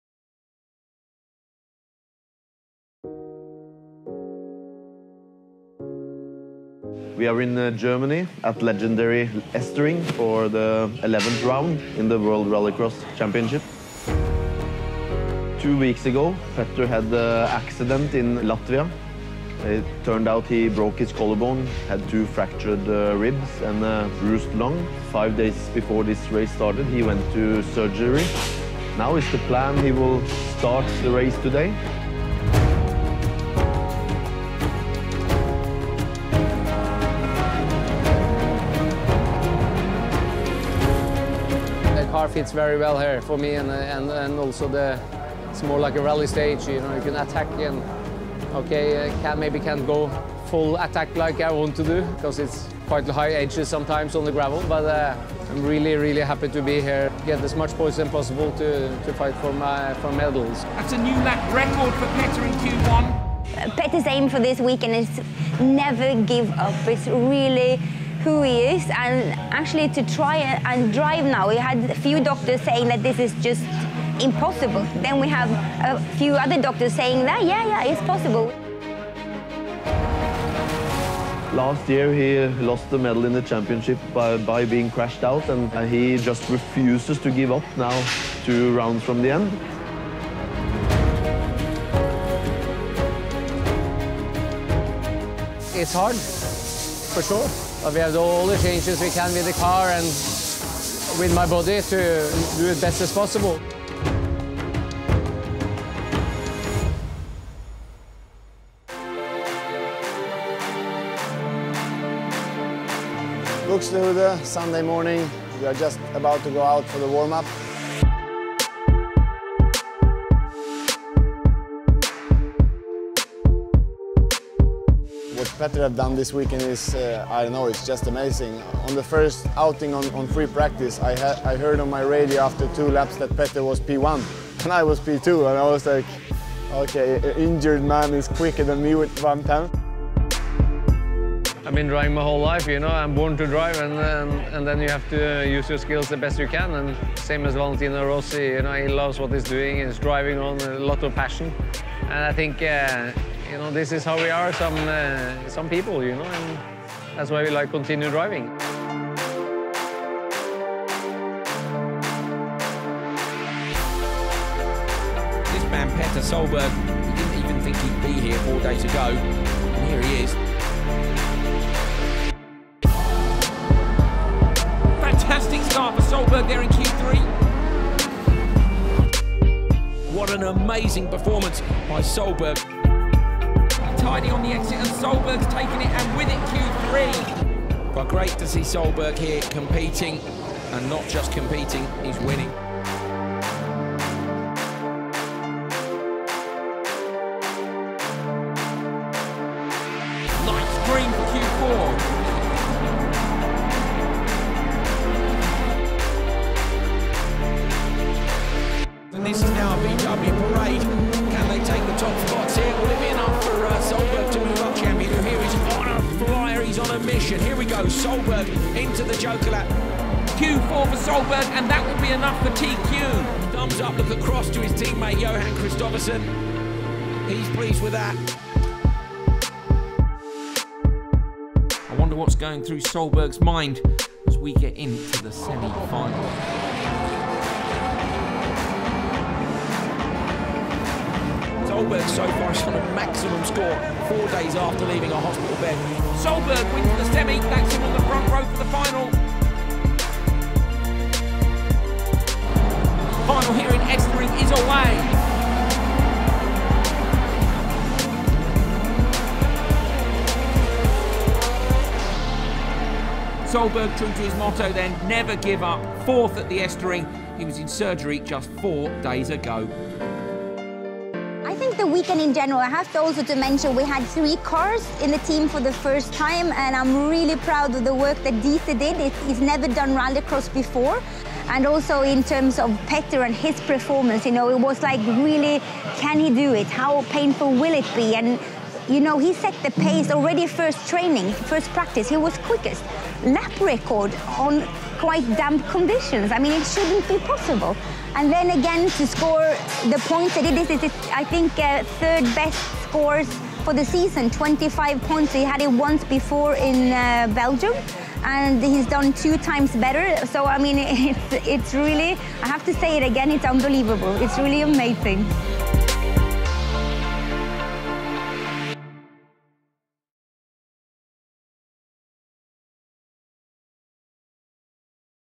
Never Give Up!
Some people said it was impossible. Even triple World Champion Petter Solberg had his doubts in the days before the race. Would he be able to compete in the FIA World RX of Germany only five days after surgery to a broken collarbone and with two fractured ribs?
One doctor gave him the vision, and Petter's lovely family and amazing team helped him carry on his self-imposed assignment.
Here's World Champion Petter Solberg's story about Never Giving Up.